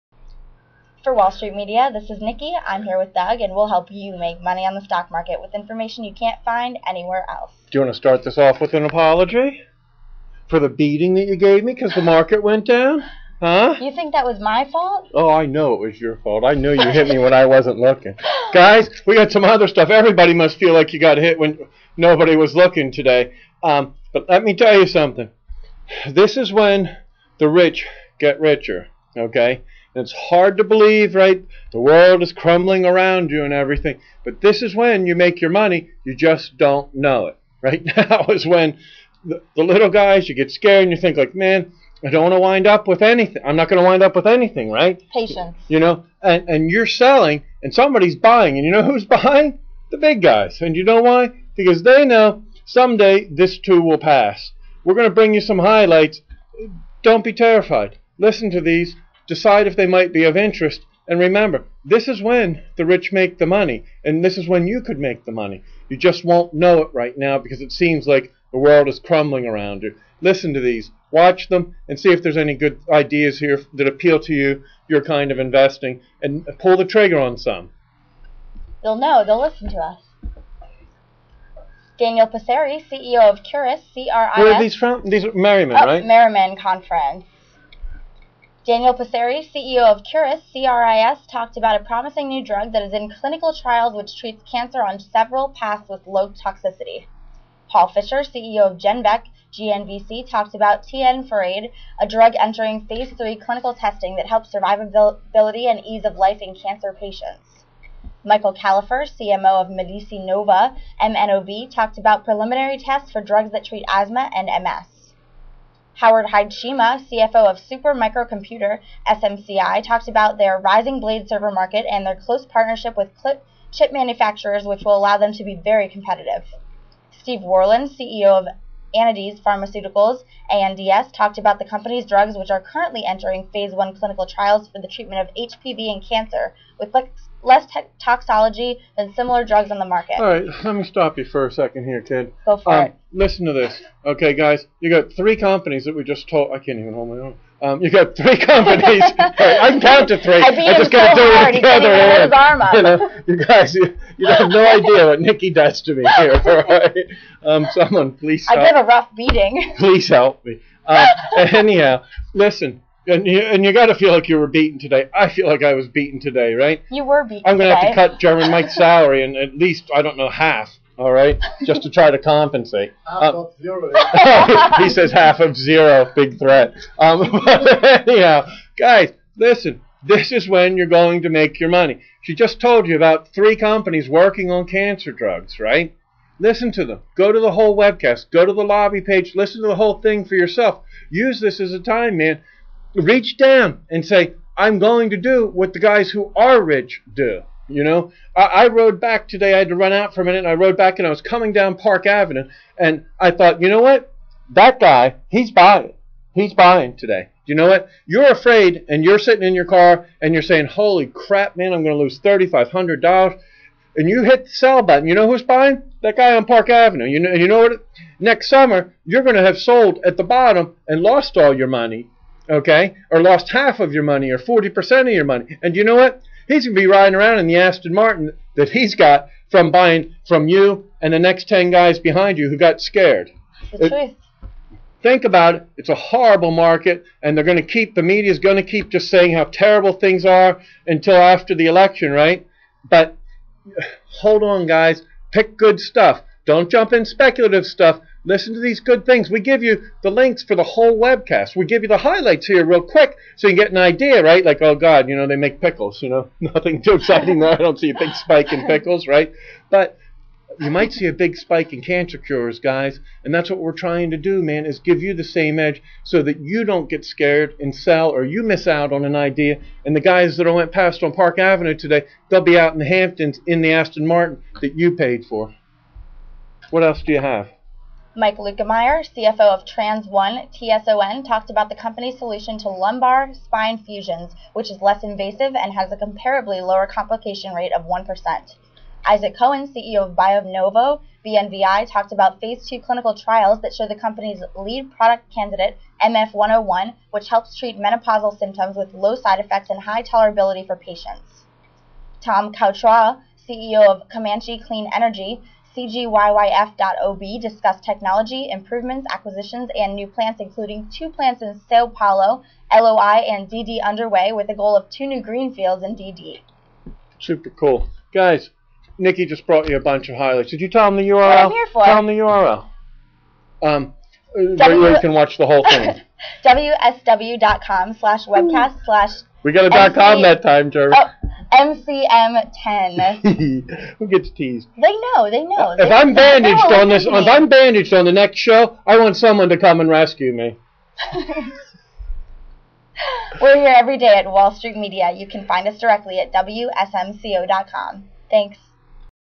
Biotech Stocks on the Move
* Merriman Curhan Ford 5th Annual Investor Summit
* Curis cancer drug in clinical trials
* GenVec cancer drug enters Phase III
* MediciNova discusses early test for asthma and MS drugs